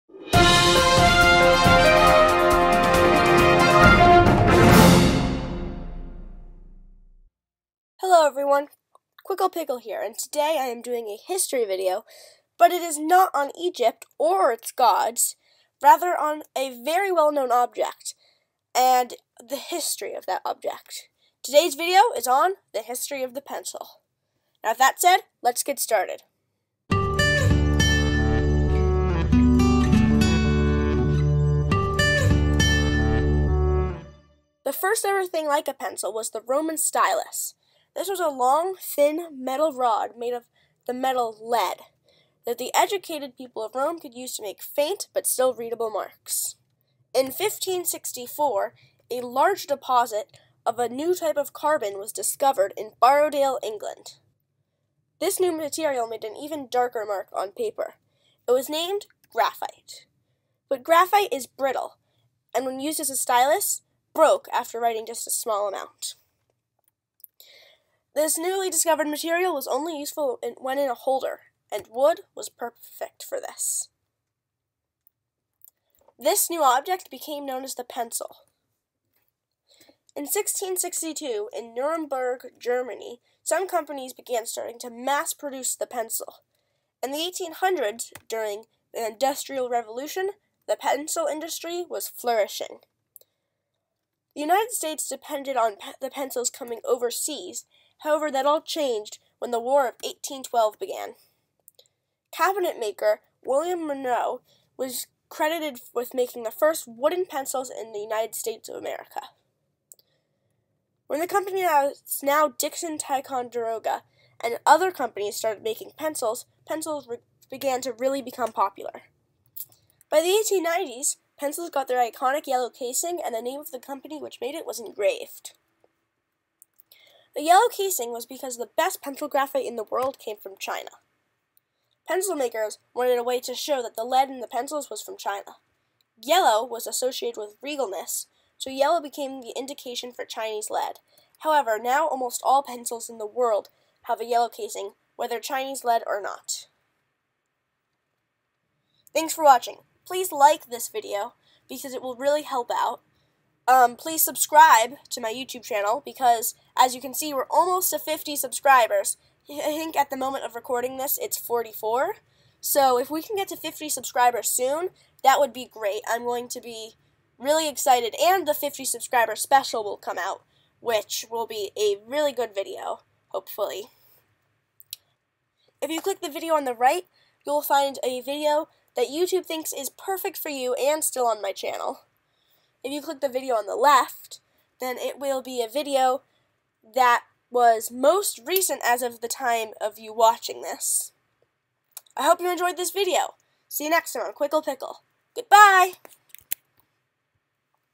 The History of the Pencil
When was the pencil invented? Why are pencils yellow? All these and more will be answered!
My Canva link, if you make a design, you get one free premium icon